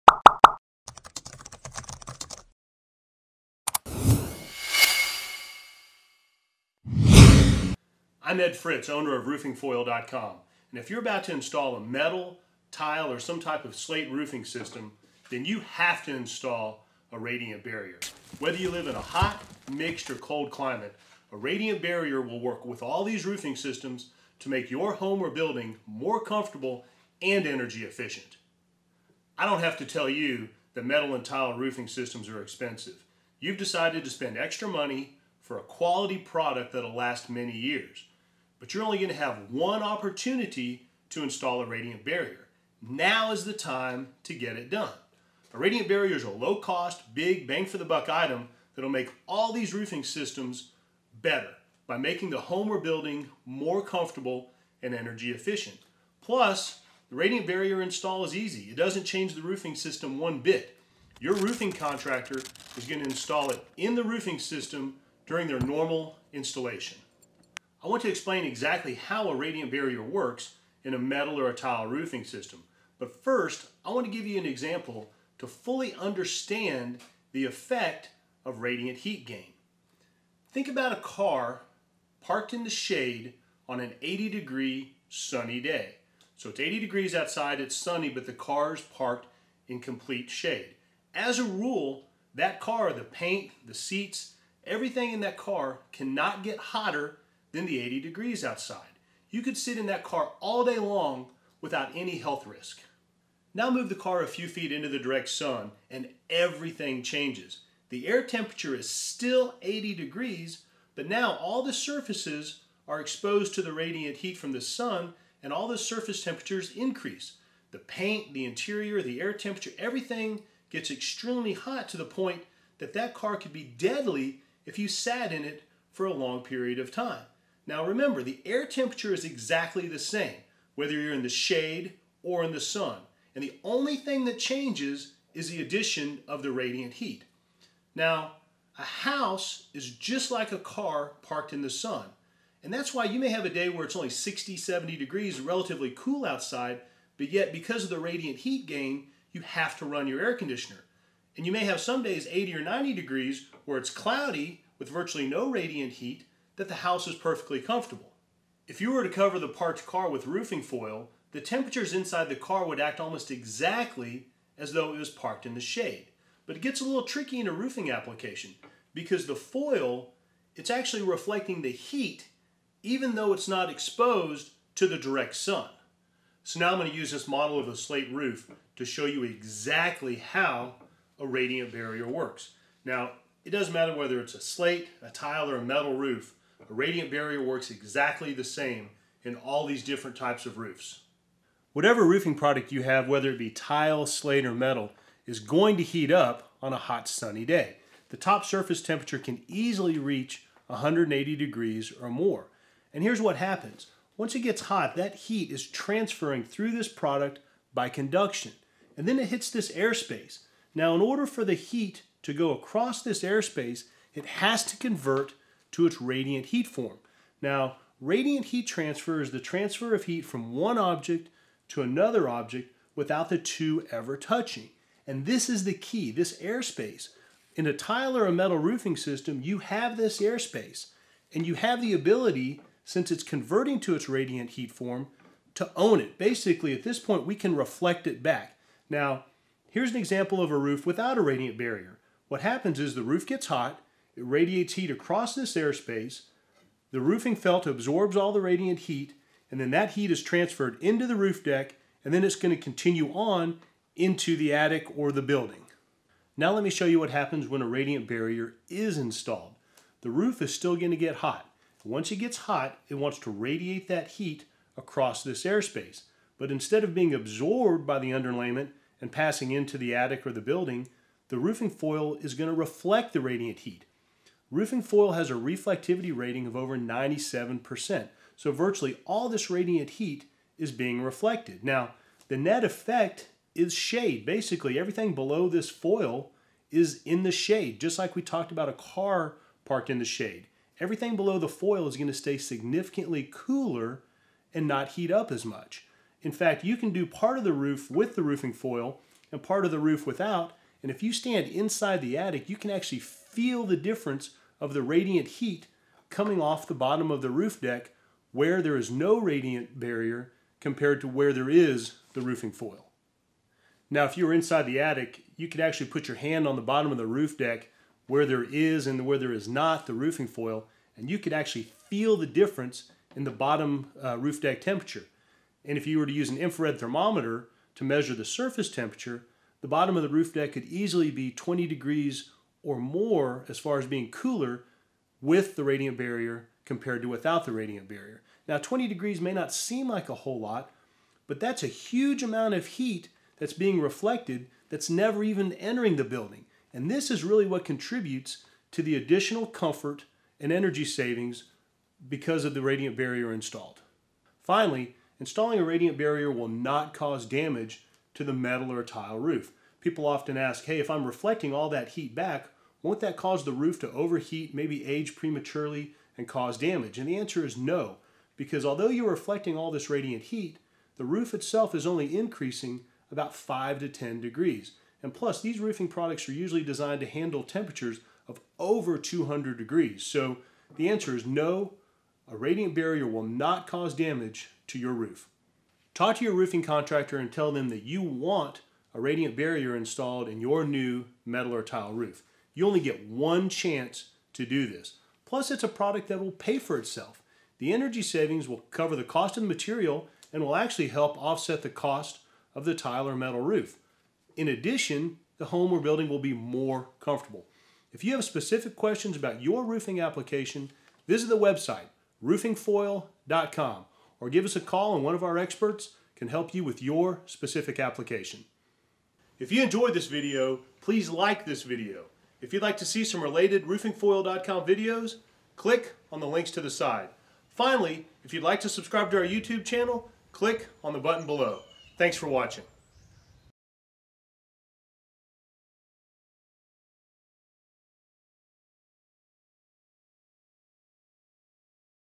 RoofingFoil - WATCH THIS BEFORE you buy a METAL or TILE ROOF!!
If you're about to install a metal, tile or some type of slate roofing system that has a built-in air space, you have to install a radiant barrier! To find out why, watch this quick video. Ed shows how easy RoofingFoil™ is to install and what a big impact installing a radiant barrier in your new roof can make on both the comfort and efficiency of your home or business.

Purchase our Miami-Dade county approved all in one underlayment AND radiant barrier